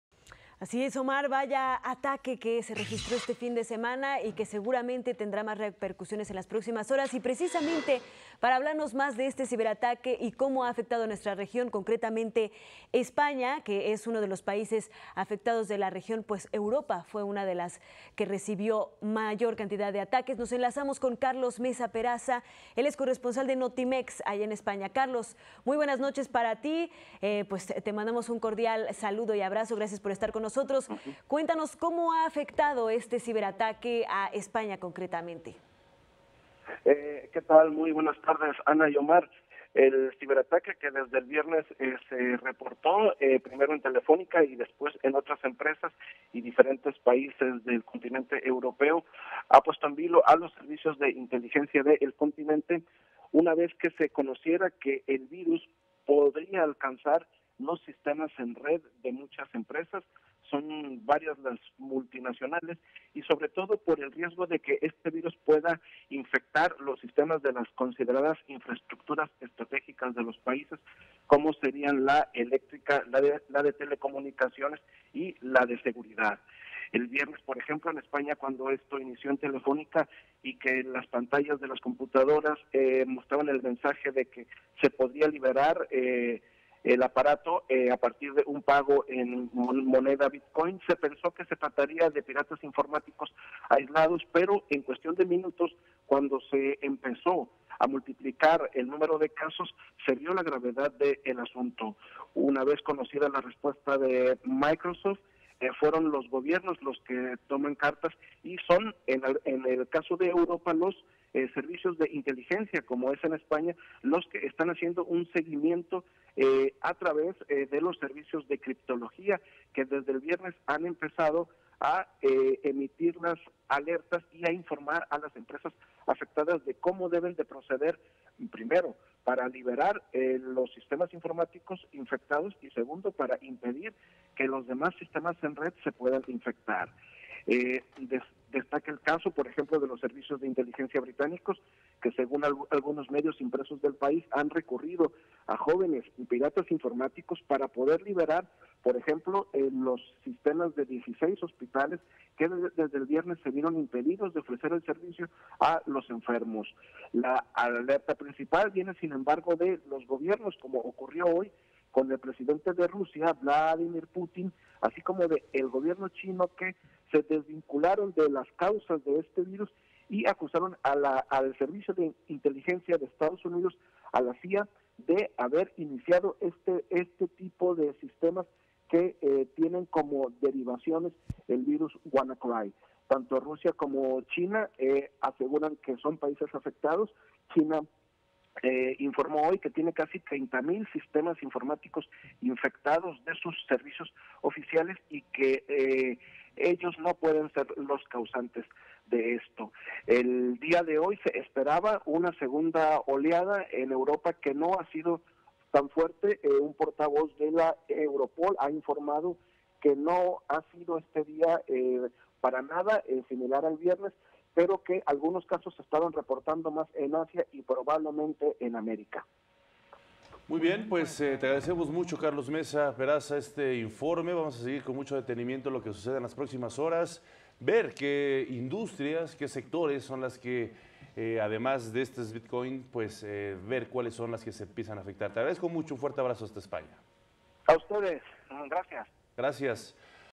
15/05/17 Carlos Meza corresponsal de Notimex en España
Enlace Para hablarnos más de este “ciberataque” a escala mundial, nos enlazamos con Carlos Meza Peraza, corresponsal de Notimex, en España.